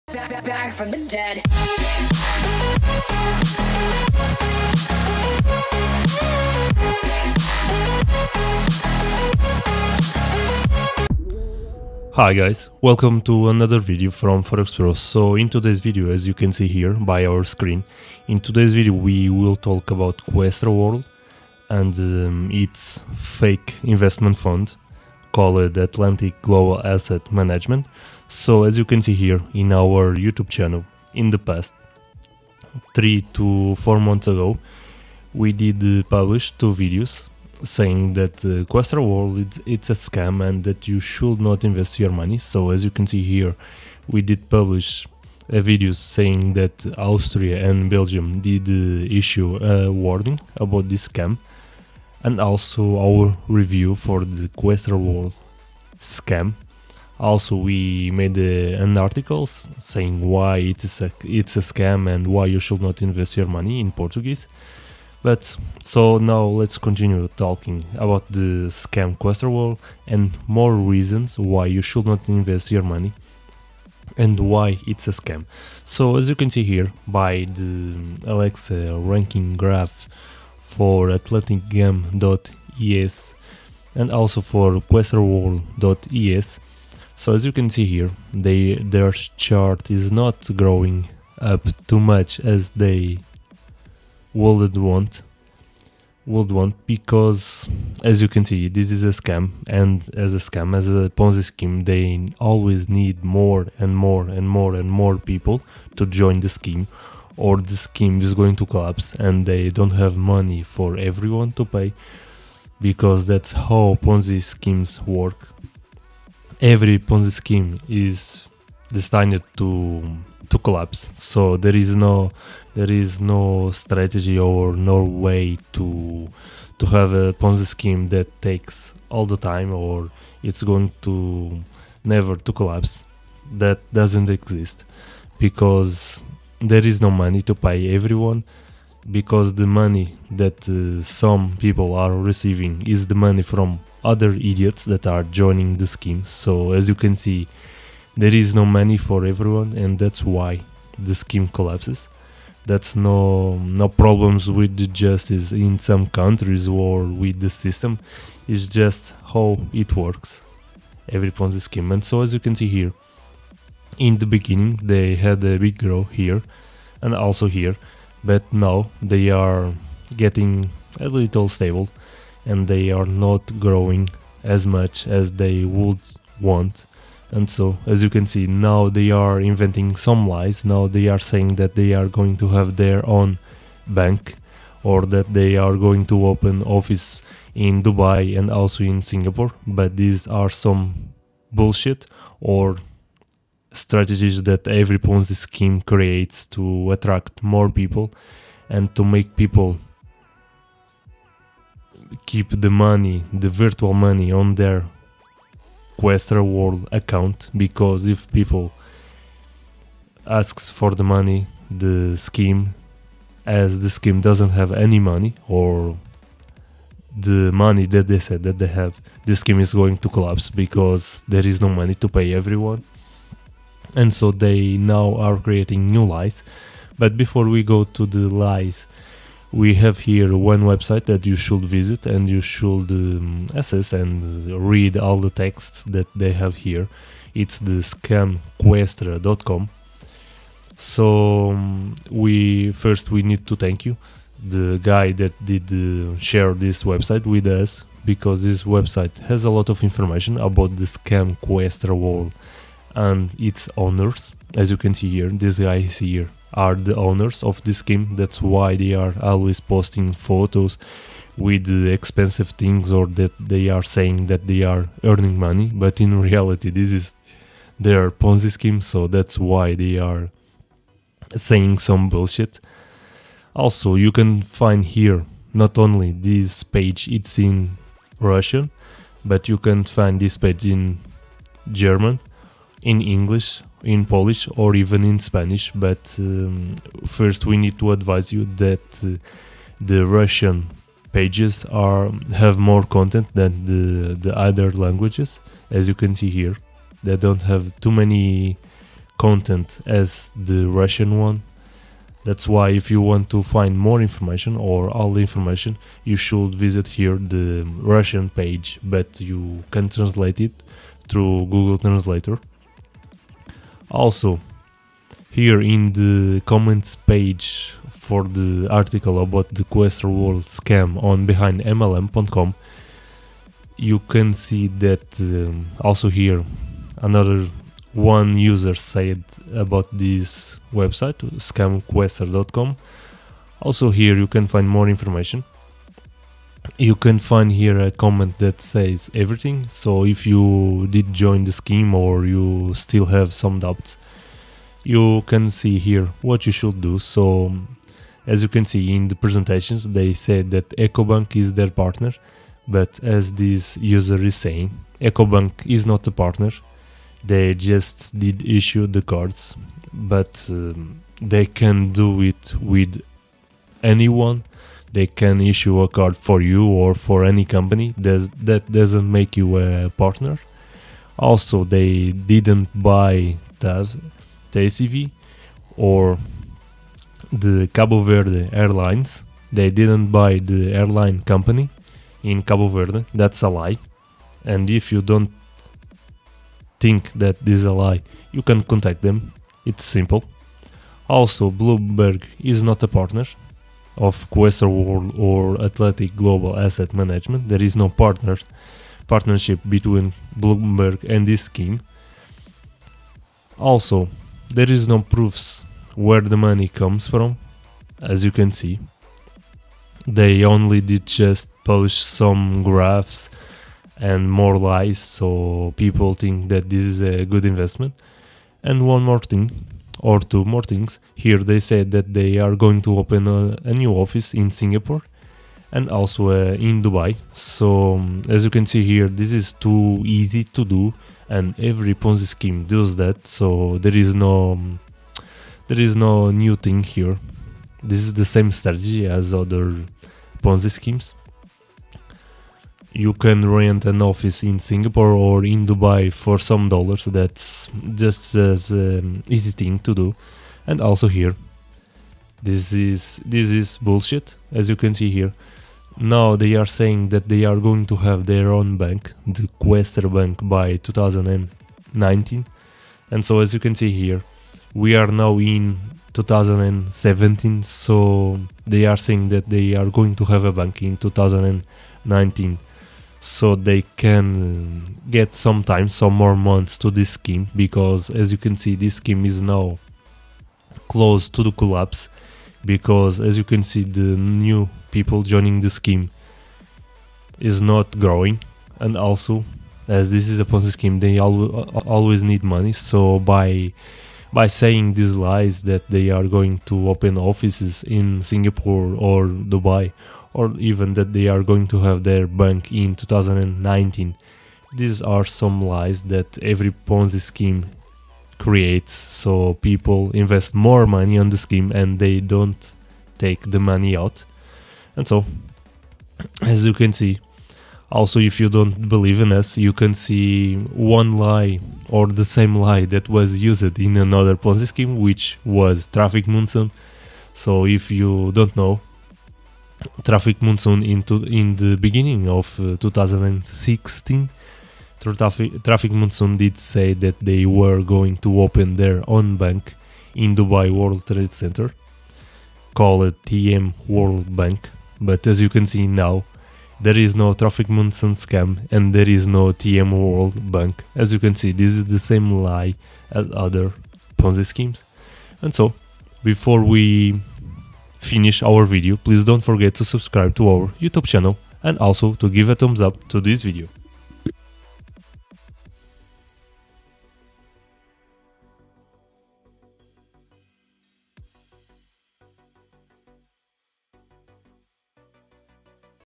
QUESTRA WORLD CREATED NEW LIES! RUSSIAN SCAMMERS WANT YOUR MONEY.
Questra World it's a Ponzi Scheme that is on our list of HYIPs and Ponzis

Questra World (and Atlantic Global Asset Managment) it's a Ponzi Scheme without any doubts.

Every Ponzi Scheme collapses when the number of new idiots decreases and it's what is happening right now with Questra World / AGAM. The number of new idiots joining the scam decreased and that's the reason why they invented that they are going to open one office in Singapore and Dubai or the Questra Bank in 2019. If you're not idiot you know that the lie of creating a Bank is used for every Ponzi scheme. Do you remember the TM Bank from the Ponzi Traffic Monsoon that never existed??

Stay away from the scam Questra World and their new bullshit!

Audio: NoCopyrightSound